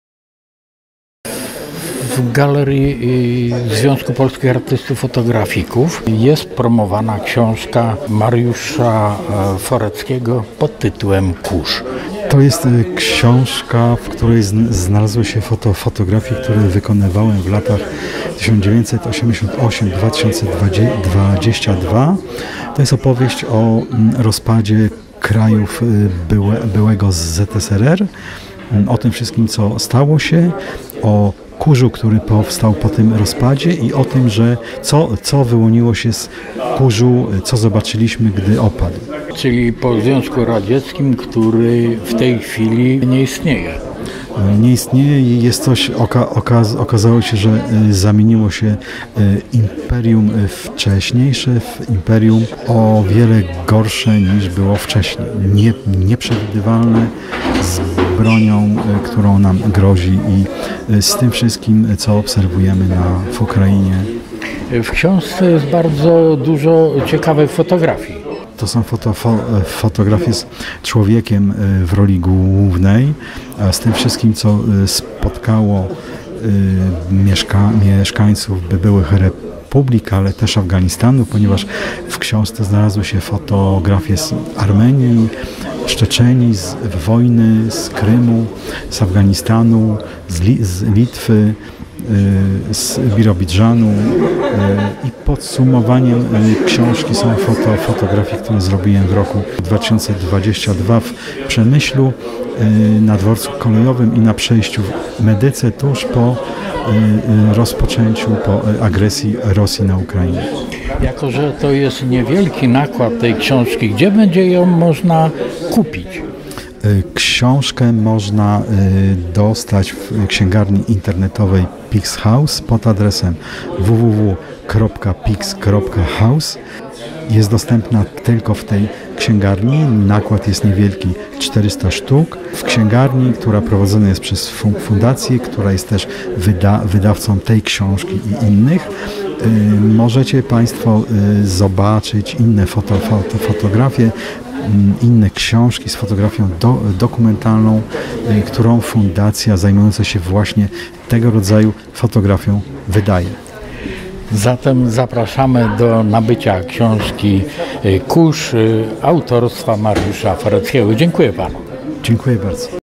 ZPAF Okręg Śląski - Mariusz Forecki Promocja książki "Kurz" - 27102022 Red. Jerzy Granowski
W ZPAF Okręg Śląski przy ul. Dąbrowskiego 2 w czwartek 27 października odbyła się premiera książki  Mariusza Foreckiego pt. "Kurz".
Mariusz Forecki (1962, dokumentalista i autor książek. Od wielu lat fotografuje człowieka uwikłanego w procesy społeczno-polityczne. Absolwent Instytutu Twórczej Fotografii Uniwersytetu Śląskiego w czeskiej Opawie, współzałożyciel Fundacji PIX.HOUSE